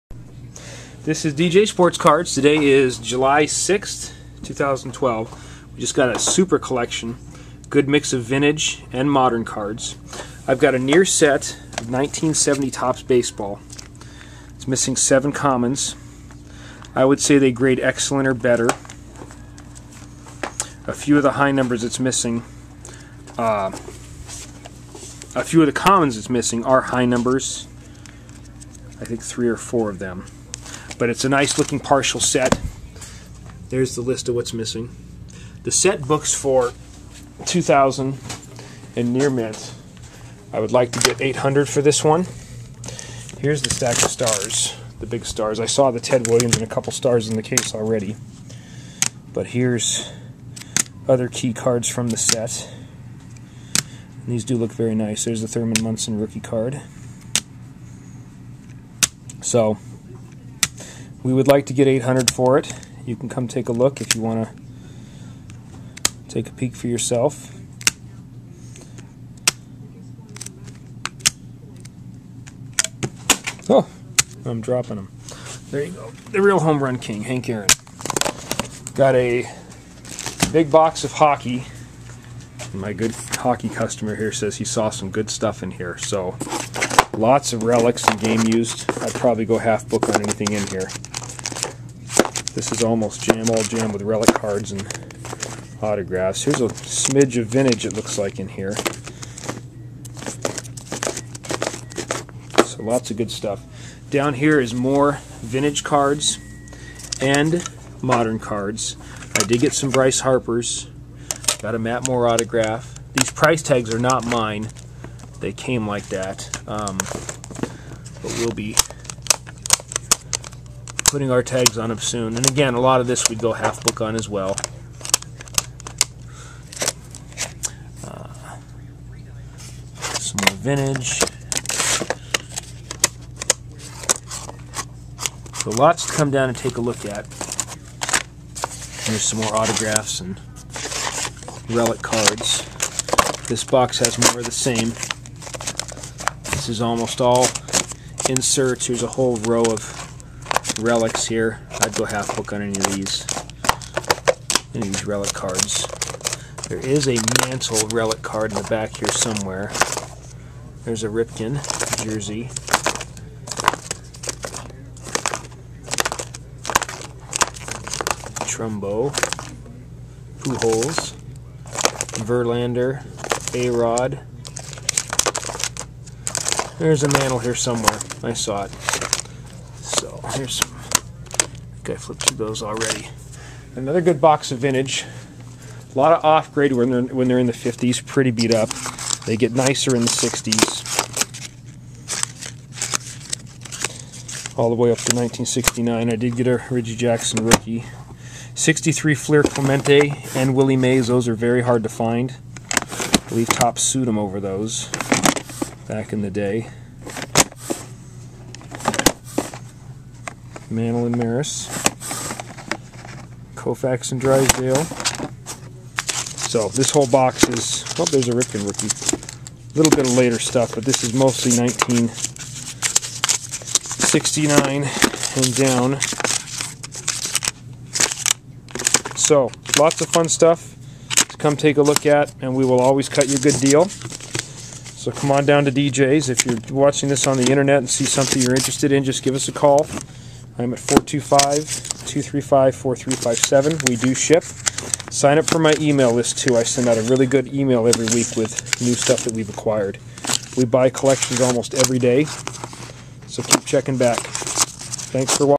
New Sports Card Collection Vintage and Modern at DJ's Sportscards Renton, WA
New collection July 6, 2012. Vintage cards, modern cards, 1970 Topps near complete set, more!